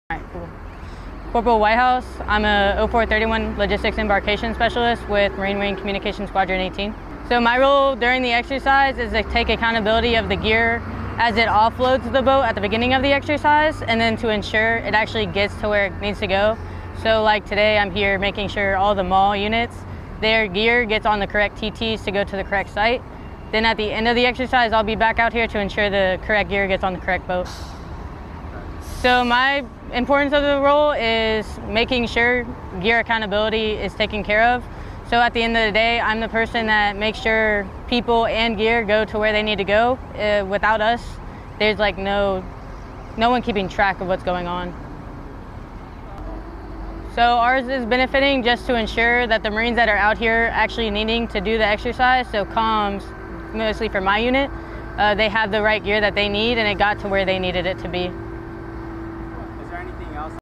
Why is the Philippines and U.S. Military Partnering for Balikatan 23?
U.S. Marine Corps Cpl. Meghan Whitehouse, a logistics embarkation specialist, with Marine Wing Communications Squadron 18, discusses her role during the preparation for Balikatan 23, in Subic Bay, Philippines, April 4, 2023.

Balikatan 23 logistics embarkation specialist Cpl. Meghan Whitehouse interview
PHILIPPINES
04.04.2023

 Balikatan is an annual exercise between the Armed Forces of the Philippines and U.S. military designed to strengthen bilateral inoperability, capabilities, trust and cooperation built over decades of shared experiences. (U.S. Marine Corps video by Lance Cpl. Manuel Rivera)

Film Credits: Video by Lance Cpl. Manuel Rivera
Exercise Balikatan